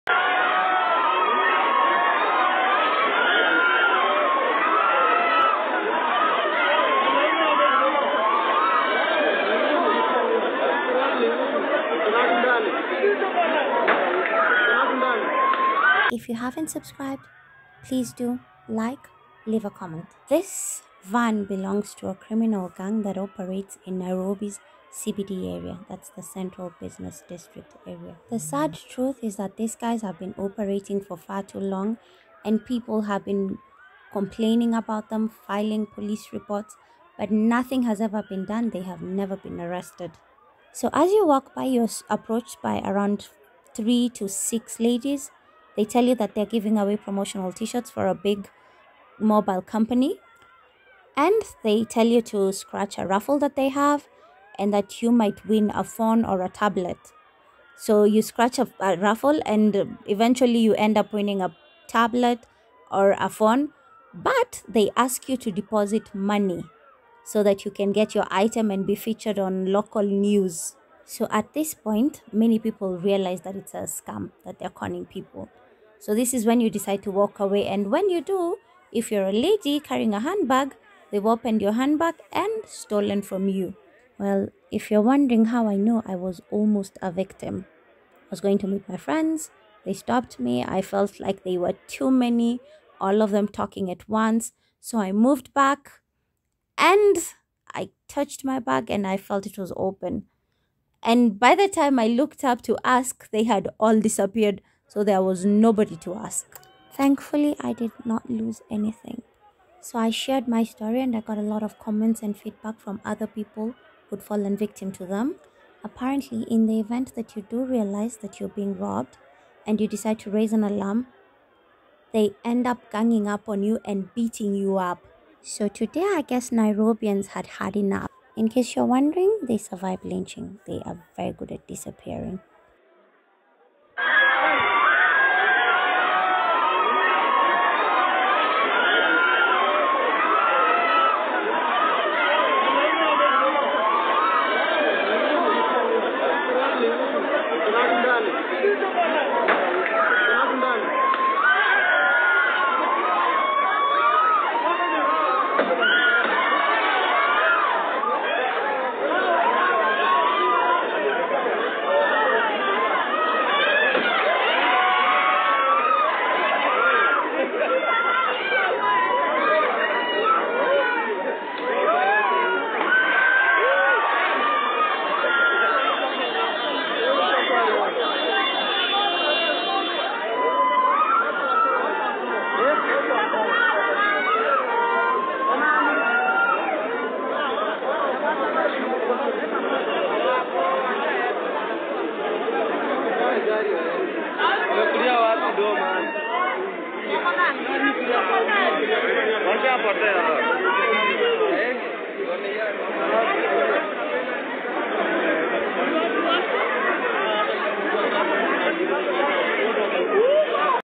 No mercy. Criminal gang escapes lynching, mob focuses fury on van
A known criminal gang was almost lynched by an angry mob after years of robbing and conning people on the streets of Nairobi.
The fles on foot leaving the vehicle they operate in.
After a failed attempt to flip the van over, the angry mob decided to vandalise the vehicle

Keeping Nairobi crime free